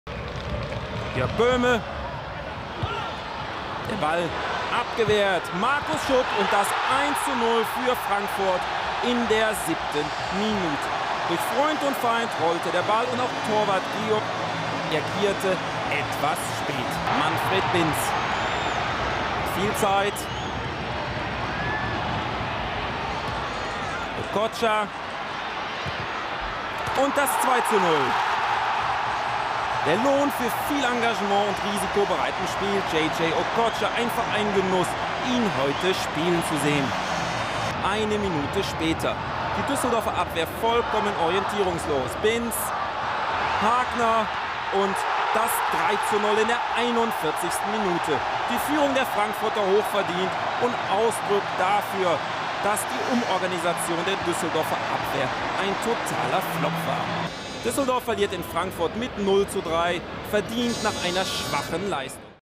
Flashback Friday I Heimsieg gegen Düsseldorf 1995
In der Saison 1995/96 gelang unserer Eintracht ein 3:0 Heimsieg gegen Düsseldorf. Der nächste Sieg folgt hoffentlich heute Abend!

Auf gehts!